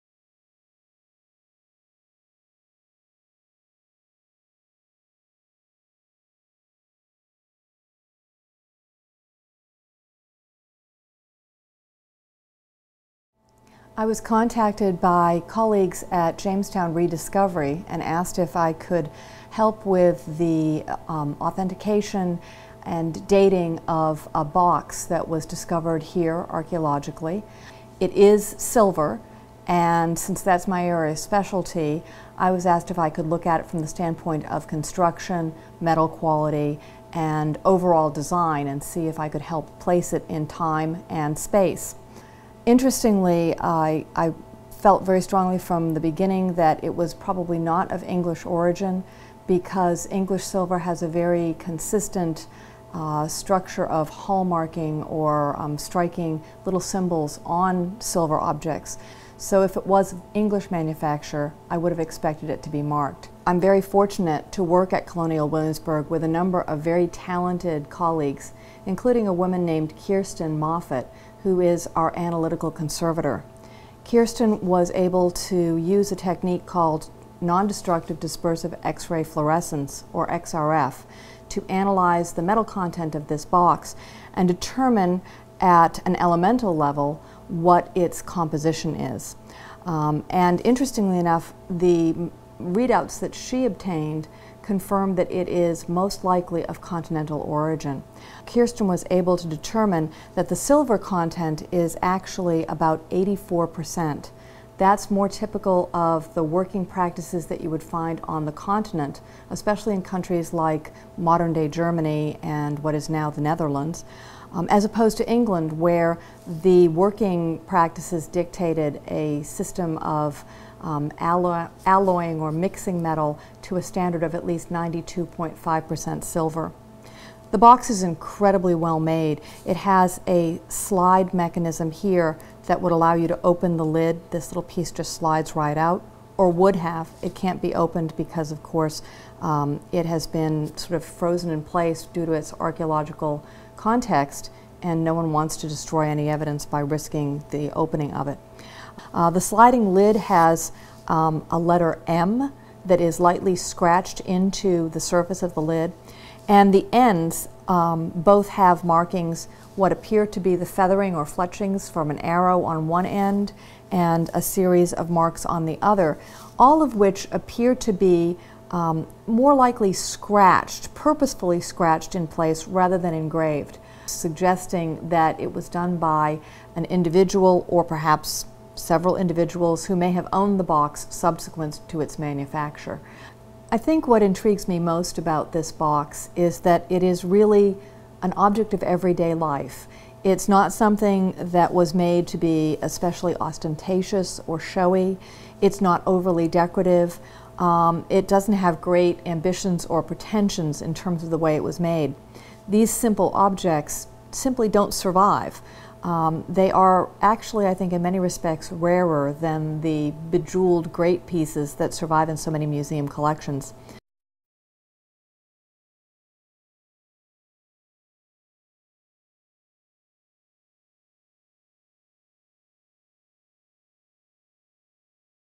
Origin of the Silver Box
In a burial within the Jamestown colony's first church, a small sealed silver box, likely a religious reliquary, was discovered. Janine Skerry, Curator of Metals, at the Colonial Williamsburg Foundation, discusses the origins of the silver box in this short film. To learn more about this and other discoveries please to see a digital model of the burial site.